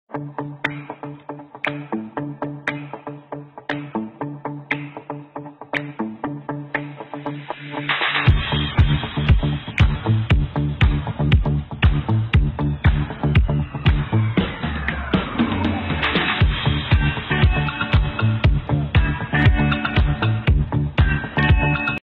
Reverstan, Matale | DJI Mavic
Slightly edited footage from a drone